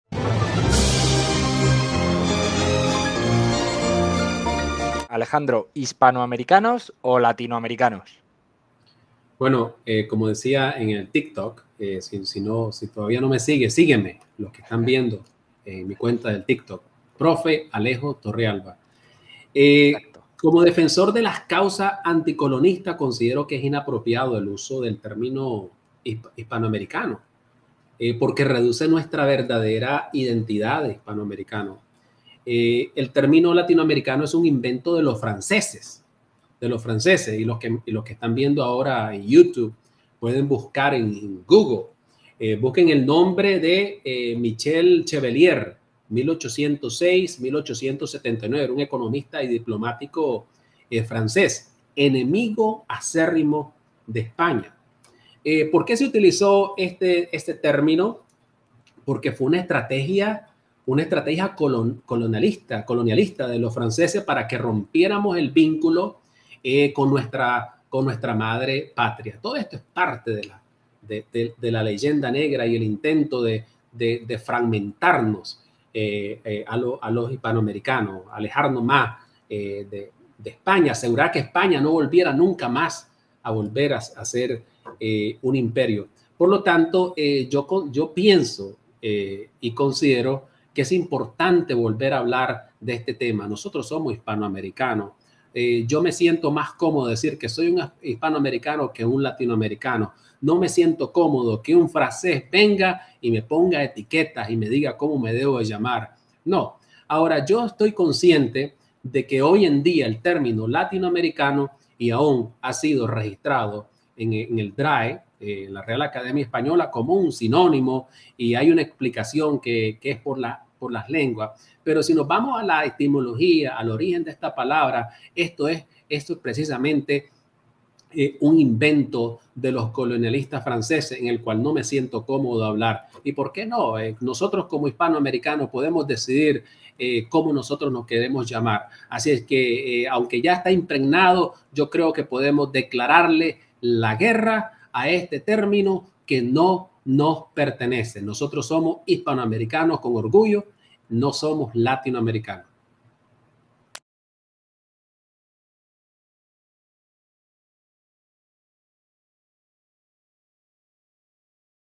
¿Hispanoamericanos o Latinoamericanos?
Fragmento de las Crónicas de Pax Hispánica  en el que entrevistamos al profesor Alejandro Torrealba. Puedes verla completa en:

Por otro lado, puedes seguirme en redes, desde donde también respondo preguntas e inquietudes de la Historia del Imperio Español: